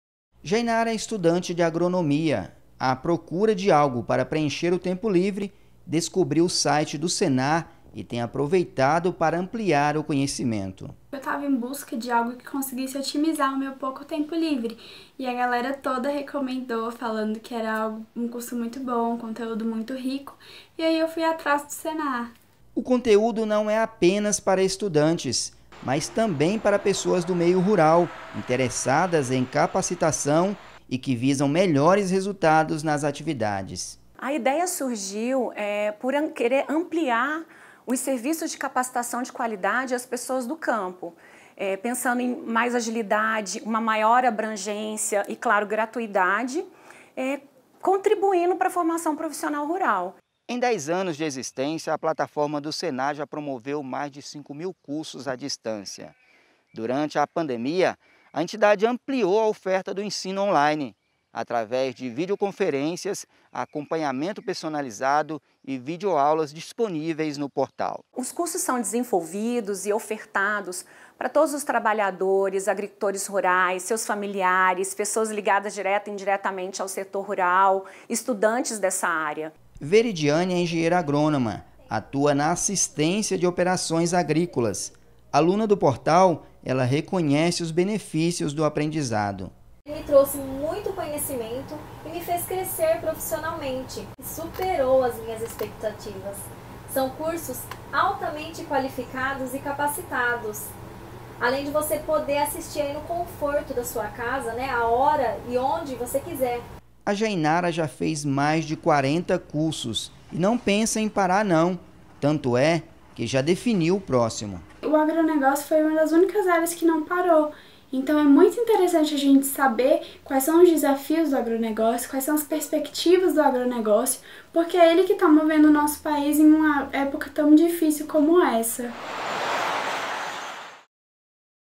Senar oferece cursos gratuitos para qualificação na área rural
Uma dica para quem procura qualificação profissional na área rural: o Serviço Nacional de Aprendizado Rural (Senar) disponibiliza de forma gratuita e on-line cursos em diversas modalidades.

Reportagem de Marcelo Tel e Messias Junqueira